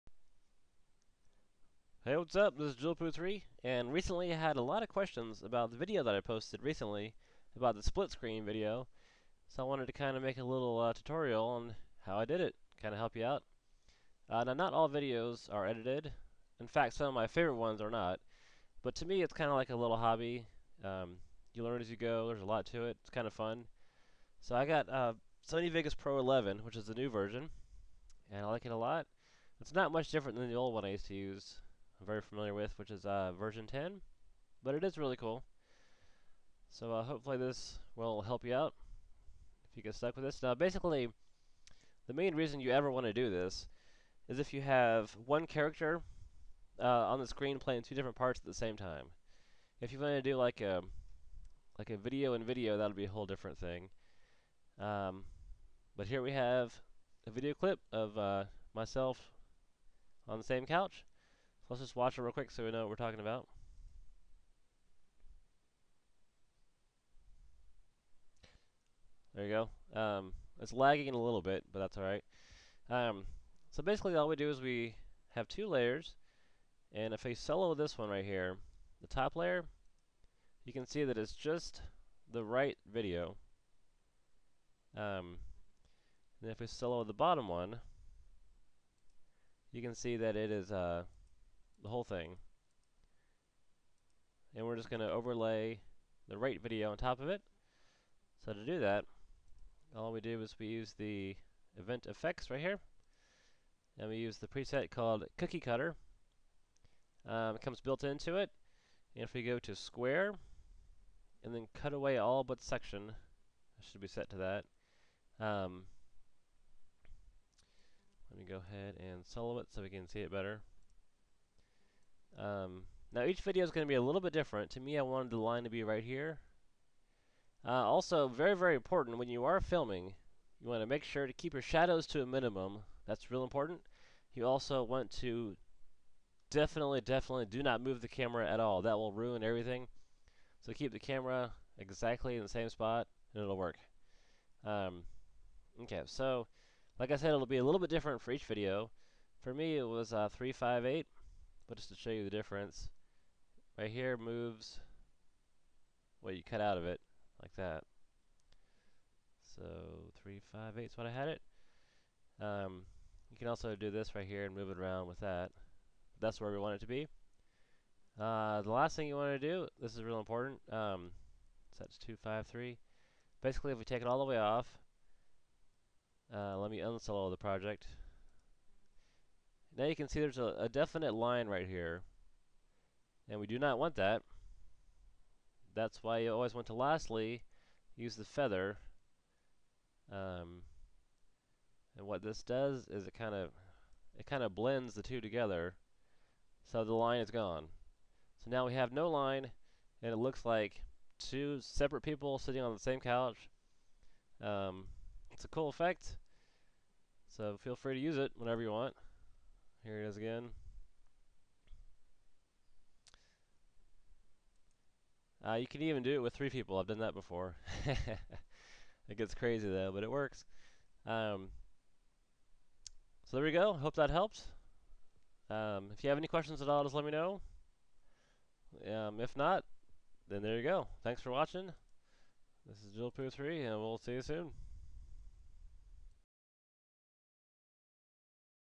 Split Screen In Sony Vegas Pro 11
This is a basic effect for having two videos meshed together as one. For example having One person on the couch talking to himself. Hope this helps and let me know if you have any questions. Happy Editing ;)